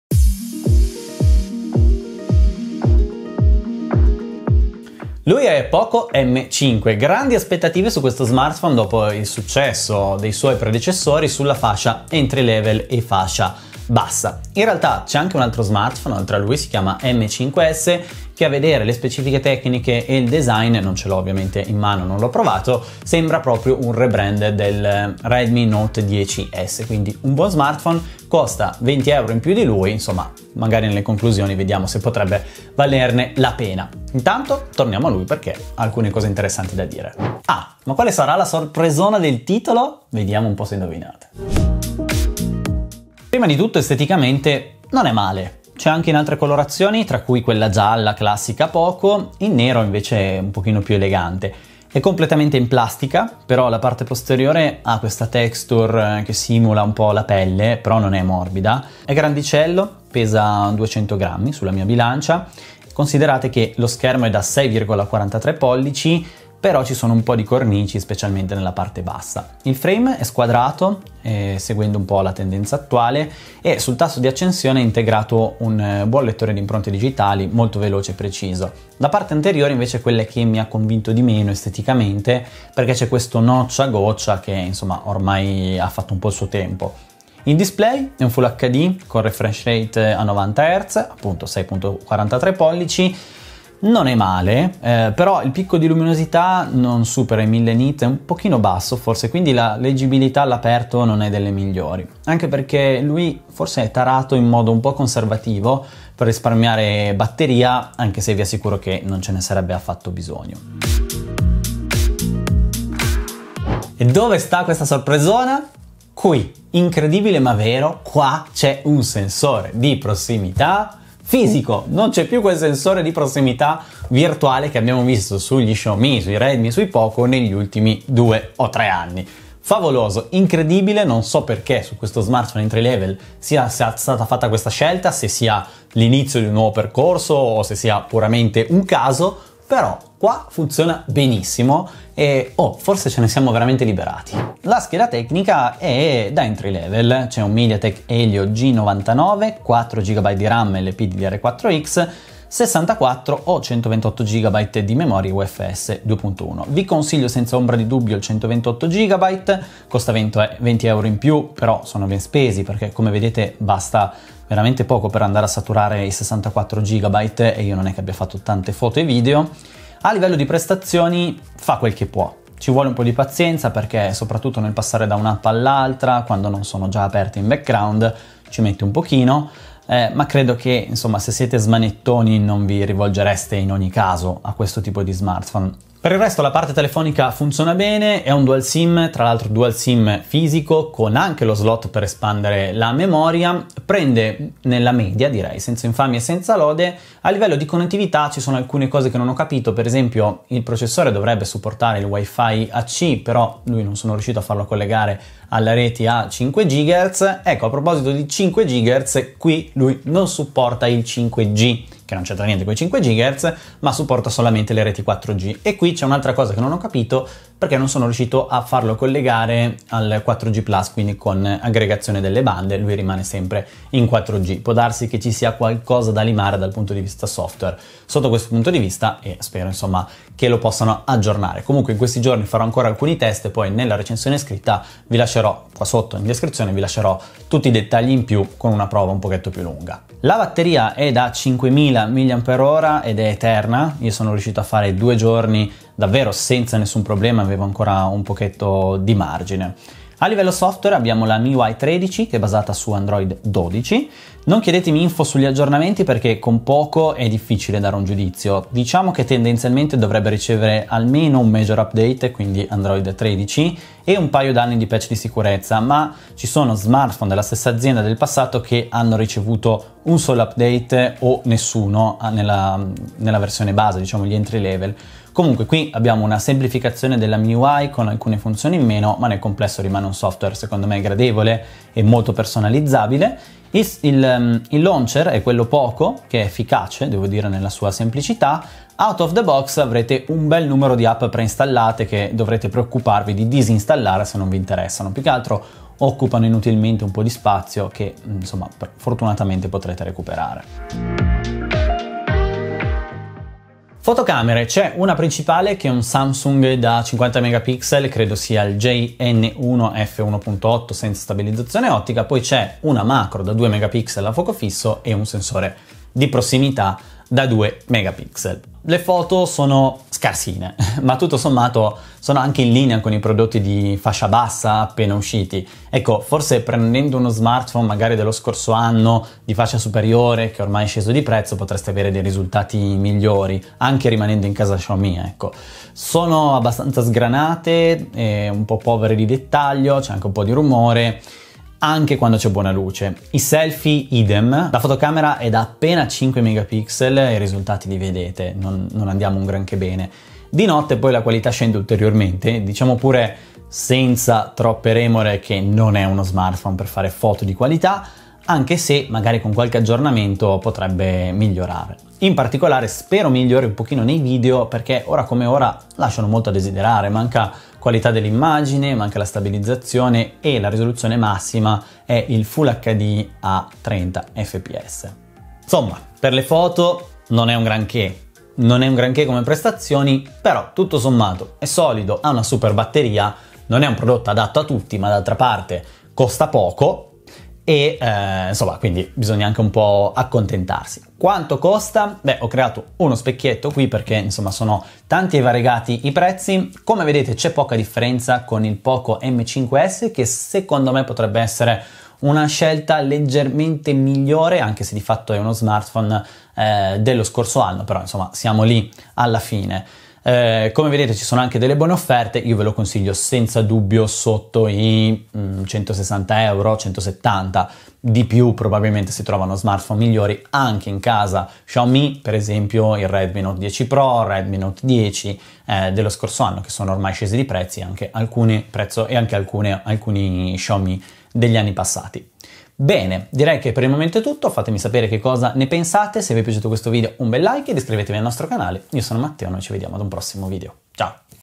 POCO M5: entry level con...SORPRESONA!
Ecco POCO M5, il nuovo smartphone entry level per chi non vuole spendere tanto ma al contempo punta ad un prodotto solido, durevole e con alcune caratteristiche tecniche affidabili. Le foto non sono un granché e le prestazioni impongono un po' di pazienza, ma nel complesso sa il fatto suo e soprattutto arriva con una novità graditissima per il mondo Xiaomi

🔥 I NOSTRI PRODOTTI TECH PREFERITI
🔥 LA NOSTRA ATTREZZATURA VIDEO